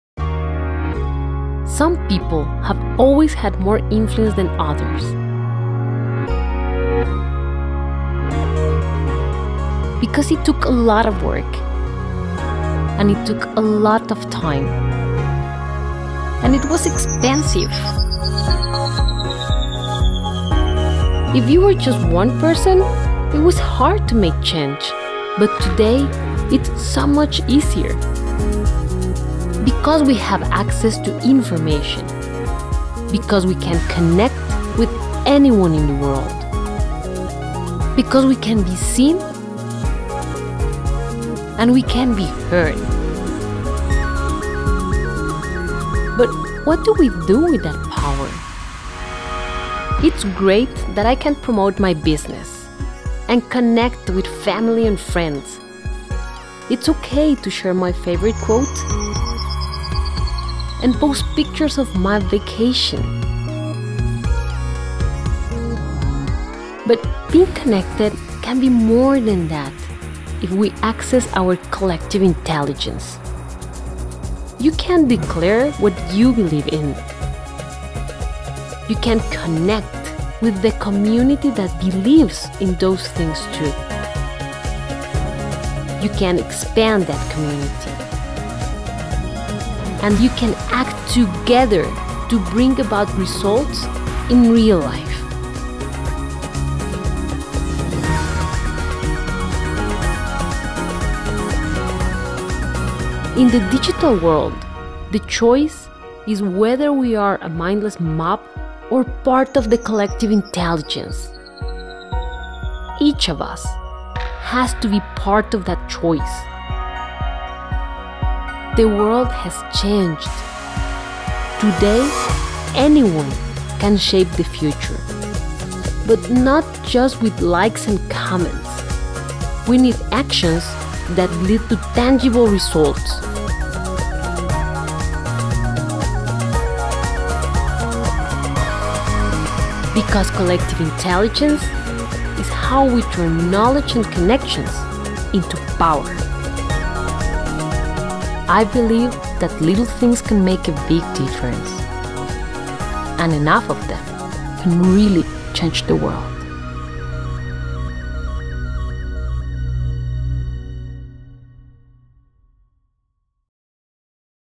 Vital Voices: Karla Ruiz Cofiño 2015
Hundredth Monkey Collective partnered with Vital Voices Global Partnership and Guatemalan digital evangelist Karla Ruiz Cofiño to create this film in support of the 2015 Global Leadership Awards. Karla is one of the recipients of the Vanguard Award, a special edition 2015 Global Leadership Award from Vital Voices Global Partnership. In this film we worked with her to create a demonstration of the concept she calls "collective intelligence," which is the power available to groups of people who focus their online activities on projects that improve the world.